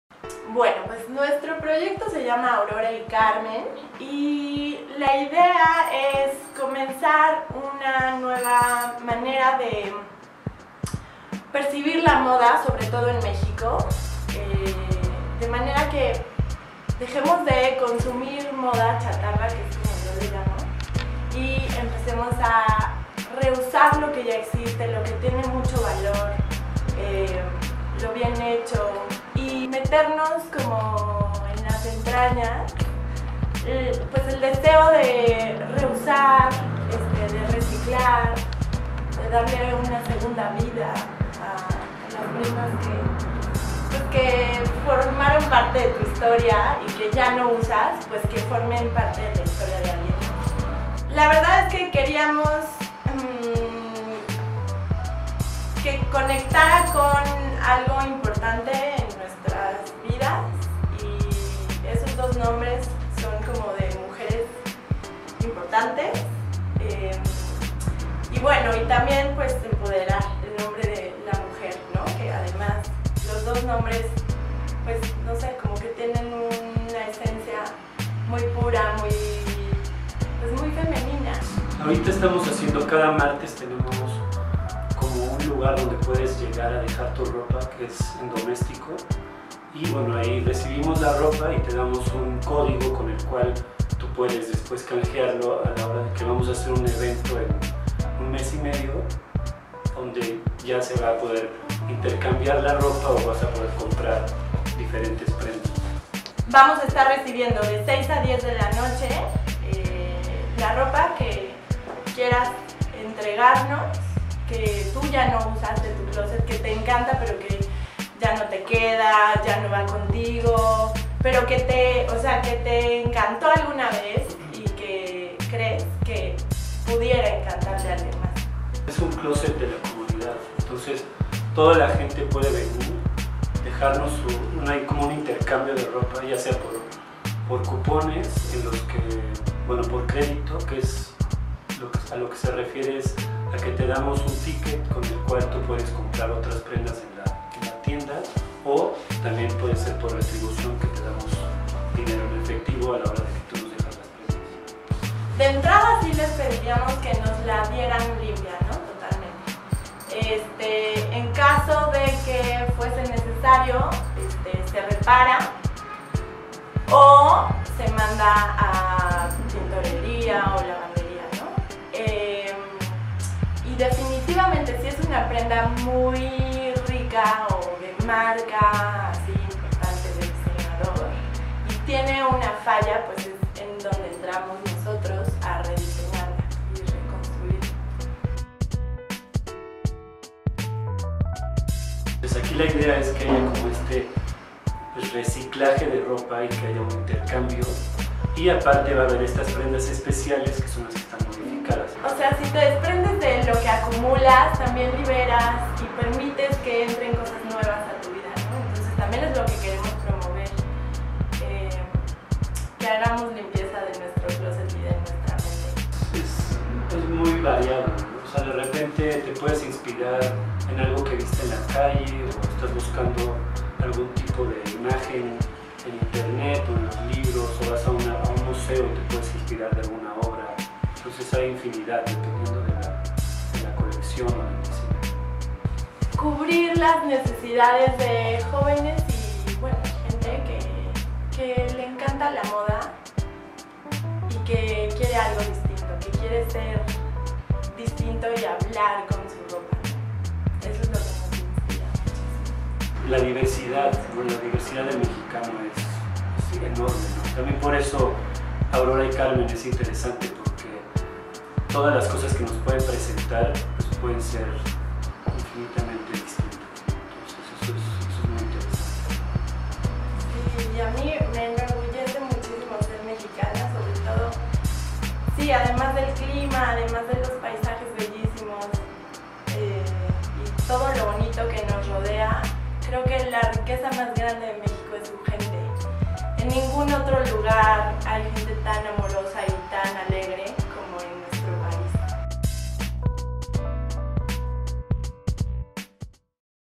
Reutilizar, reciclar y darle un nuevo uso a la ropa con Aurora y Carmen
Aurora y Carmen nació con un solo propósito, darle un segundo uso a la ropa, en buen estado, o poco desgastada, y con ello romper los estereotipos de la moda.